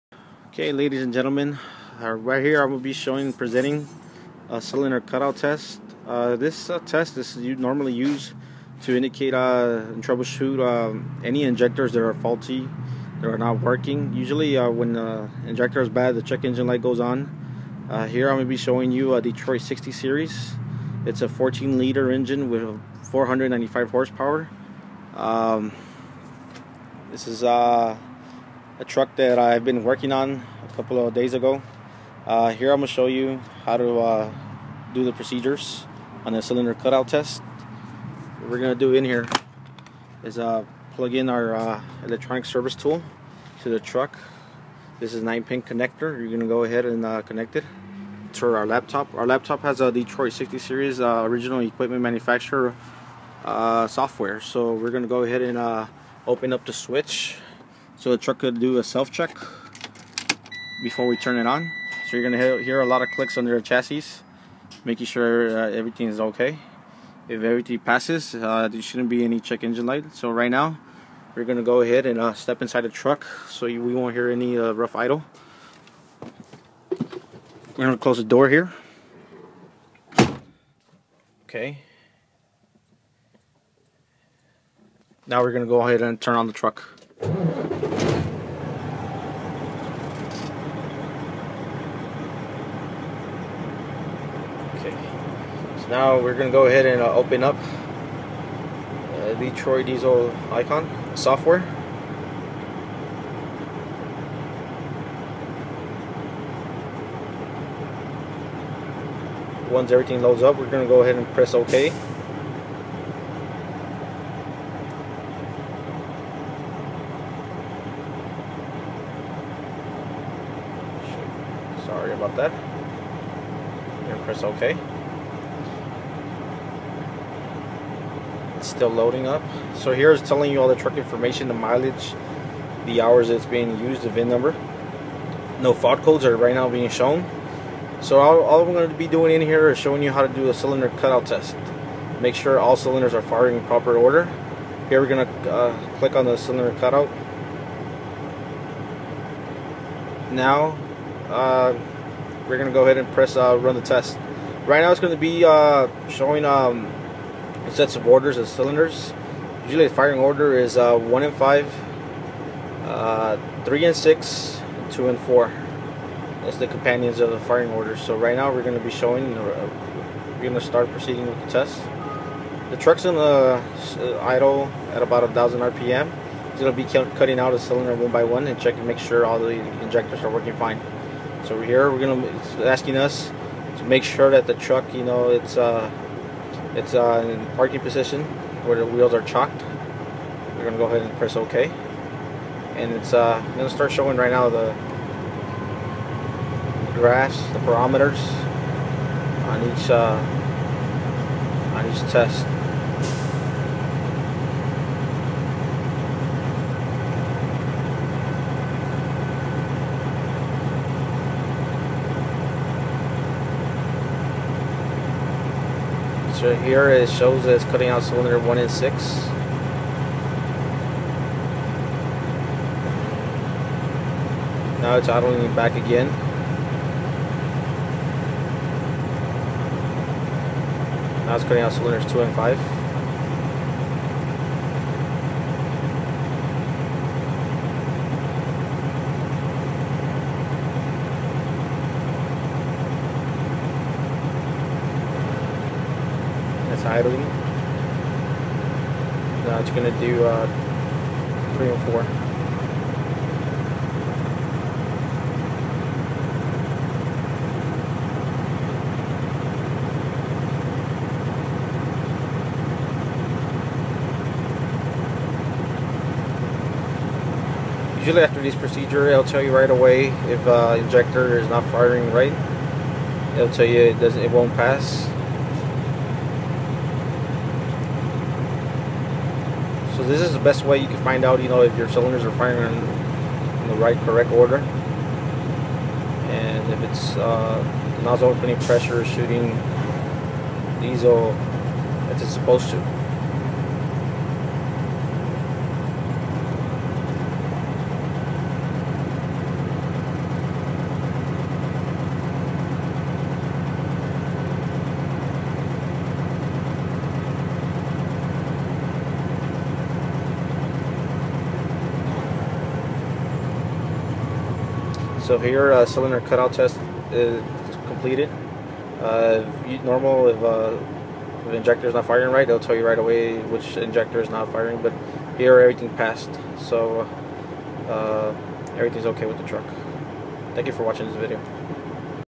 Detroit diesel 60 series cylinder cut out test
Checking for faulty injectors